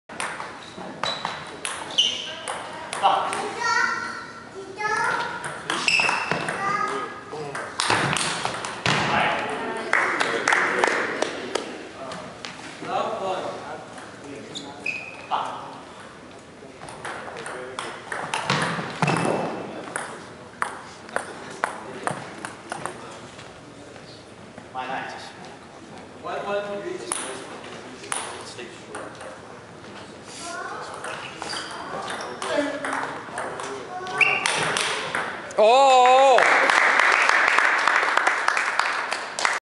Rocky Wang Vs Yuchi Kageyama, Knoxville, Tennessee, August 2011, Part 1 Game 4 of 7
Rocky Wang Vs Yuchi Kageyama, Knoxville, Tennessee, August 2011, part one Game 1 of 7. Sorry ran out of memory